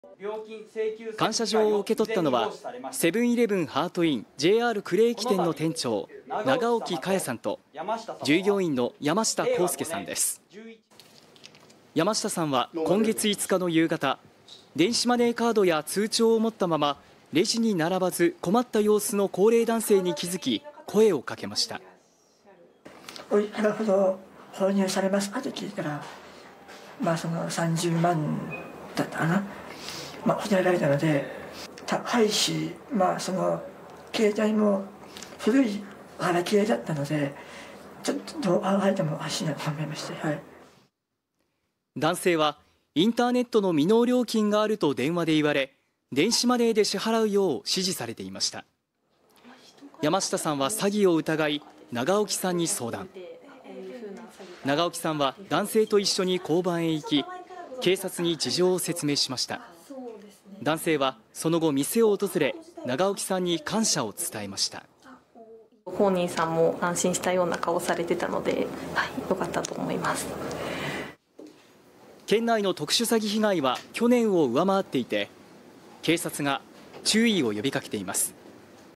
コンビニ店長と従業員に感謝状 高齢男性に声をかけ特殊詐欺の被害防ぐ 広島・呉市
特殊詐欺の被害に遭いそうになっていた高齢の男性に声をかけ被害を防止したとして警察がコンビニの店長と従業員に感謝状を贈りました。

感謝状を受け取ったのは、セブン‐イレブンハートインＪＲ呉駅店の店長、長沖茅さんと従業員の山下康輔さんです。
山下さんは今月５日の夕方、電子マネーカードや通帳を持ったまま、レジに並ばず困った様子の高齢男性に気づき、声をかけました。

【山下康輔さん】
「『おいくらほど購入されますか？』と聞いたら、３０万と答えたので、高いし、携帯も古いガラケーだったので、ちょっとどう考えてもおかしいなと思いました」

男性は、インターネットの未納料金があると電話で言われ、電子マネーで支払うよう指示されていました。
山下さんは詐欺を疑い、長沖さんに相談。長沖さんは、男性と一緒に交番へ行き、警察に事情を説明しました。
男性はその後、店を訪れ、長沖さんに感謝を伝えました。

【長沖茅さん】
「ご本人さんも安心したような顔をされていたので、良かったと思います」

県内の特殊詐欺被害は去年を上回っていて県警が注意を呼び掛けています。